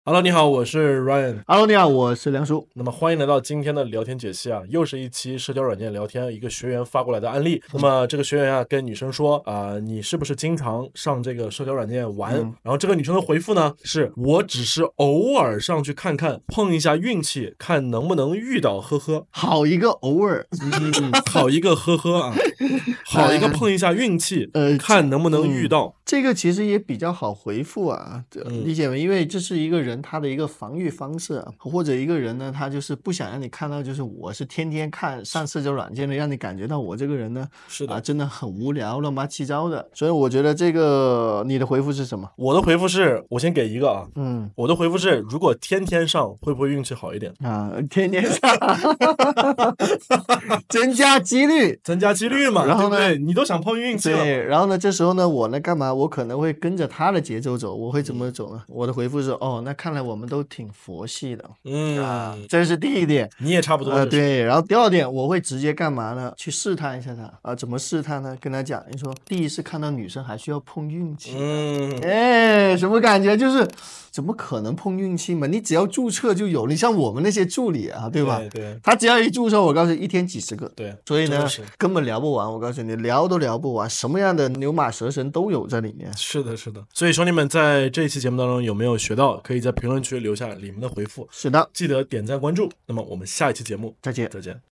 女生說偶爾玩社交軟件，該怎麽回復。瑞恩幫你戀愛聊天解析
瑞恩微信：ryan91mb
瑞恩LINE：ryan91cc